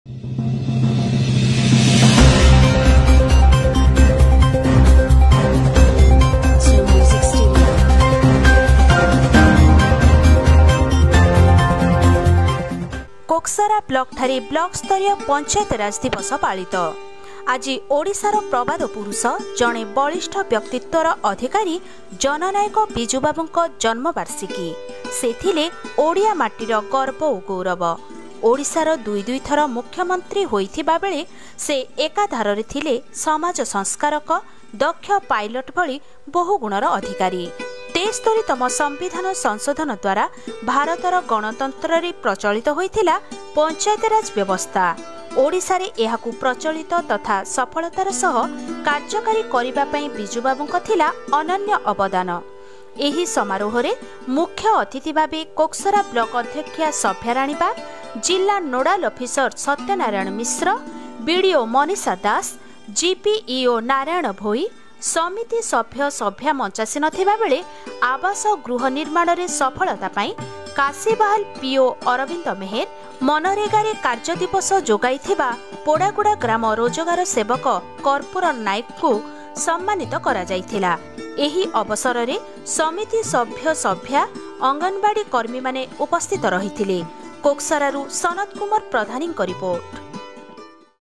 Panchayati Raj day observed in koksara
ଆଜି ଓଡ଼ିଶାର  ପ୍ରବାଦ ପୁରୁଷ,ଜଣେ ବଳିଷ୍ଠ ବ୍ୟକ୍ତିତ୍ୱର ଅଧିକାରୀ ଜନନାୟକ ବିଜୁବାବୁଙ୍କ ଜନ୍ମ ବର୍ଷିକୀ ସେ ଥିଲେ ଓଡ଼ିଆ ମାଟିର ଗର୍ବ ଓ ଗୌରବ ଓଡ଼ିଶାର ଦୁଇ ଦୁଇ ଥର ମୁଖ୍ୟମନ୍ତ୍ରୀ ହୋଇଥିବା ବେଳେ ସେ ଏକାଧାରରେ ଥିଲେ ସମାଜ ସଂସ୍କାରକ, ଦକ୍ଷ ପାଇଲଟ ଭଳି ବହୁଗୁଣର ଅଧିକାରୀ ୭୩ତମ ସମ୍ବିଧାନ ସଂଶୋଧନ ଦ୍ୱାରା ଭାରତର ଗଣତନ୍ତ୍ରରେ ପ୍ରଚଳିତ ହୋଇଥିଲା ପଞ୍ଚାୟତରାଜ ବ୍ୟବସ୍ଥା ଓଡ଼ିଶାର ଏହାକୁ ପ୍ରଚଳିତ ତଥା ସଫଳତାର ସହ କାର୍ଯ୍ୟକାରୀ କରିବା ପାଇଁ ବିଜୁବାବୁଙ୍କ ଥିଲା ଅନନ୍ୟ ଅବଦାନ  ଏହି ସମାରୋହ ରେ ମୁଖ୍ୟ ଅତିଥି ଭାବେ କୋକସରା ବ୍ଳକ ଅଧ୍ୟକ୍ଷ୍ୟା ଶବ୍ୟାରାଣୀ ବାଗ ଜିଲ୍ଲା ନୋଡାଲ ଅଫିସର ସତ୍ୟନାରାୟଣ ମିଶ୍ର ବିଡ଼ିଓ ମନୀଷା ଦାସ , GPEO ନାରାୟଣ ଭୋଇ ସମିତି ସଭ୍ୟସଭ୍ୟା ମଞ୍ଚାସିନ ଥିବାବେଳେ ଆବାସ ଗୃହ ନିର୍ମାଣ ରେ ସଫଳତା ପାଇଁ କାଶିବାହଲ ପିଇଓ ଅରବିନ୍ଦ ମେହେର, ମନରେଗା ରେ କାର୍ଯ୍ୟ ଦିବସ ଯୋଗାଇ ଥିବା ପୋଡ଼ାଗୁଡା ଗ୍ରାମ ରୋଜଗାର ସେବକ କର୍ପୁର ନାଏକ ଙ୍କୁ ସମ୍ମାନିତ କରାଯାଇଥିଲା, ଏହି ଅବସରରେ ସମିତି ସଭ୍ୟ ସଭ୍ୟା ଅଙ୍ଗନବାଡି କର୍ମି ମାନେ ଉପସ୍ଥିତ ରହିଥିଲେ